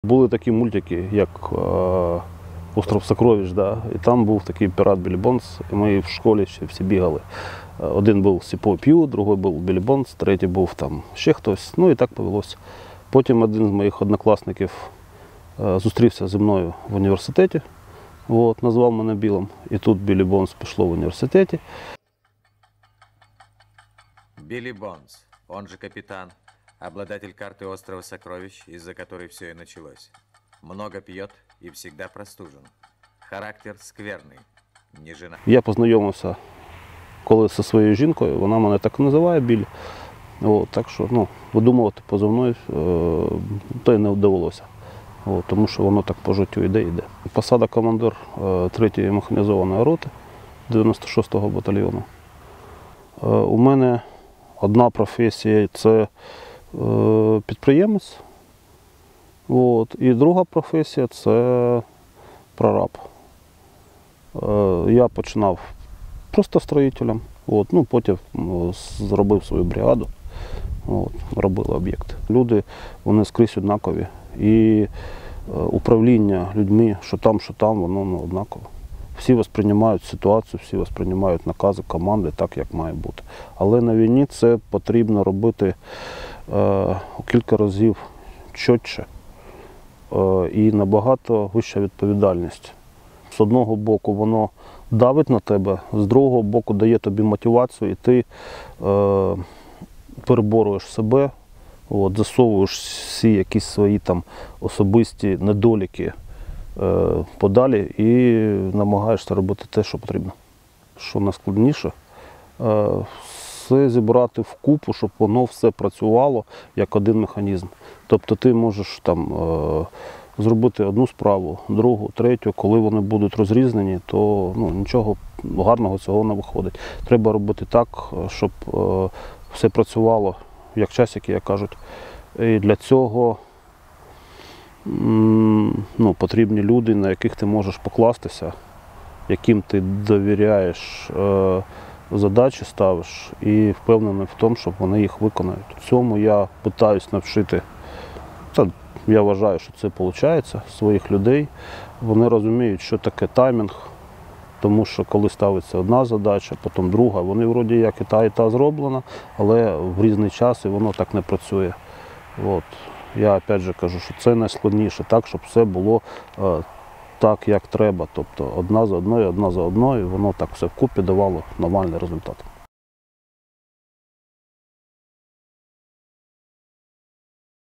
"Білл" про важливість своєчасного виконання завдань
"Білл", який у мирному житті працював виконробом на будівництві, знайшов нове застосування своїм навичкам на фронті. Він знає, що вчасно виконане завдання та надійні побратими – це ключ до успіху як у цивільному, так і у військовому житті. Його досвід і вміння організовувати людей, дотримуватись таймінгів та розв'язувати проблеми допомагають йому і зараз. Сила команди – в єдності, це зараз головне!